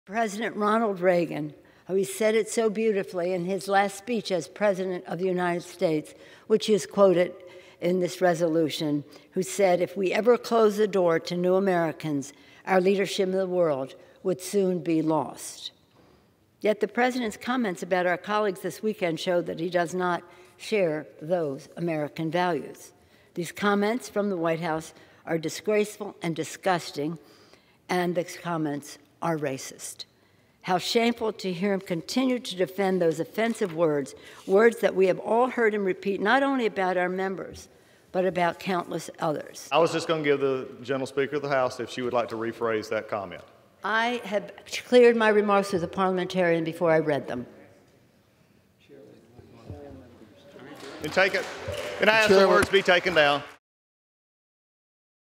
Nancy Pelosi: President Donald Trump's 'comments are racist' | USA TODAY
Nancy Pelosi calls out President Donald Trump for 'racist' comments on House floor.

Speaker of the House Nancy Pelosi responded to the recent comments made by President Donald Trump and the White House while on the House floor.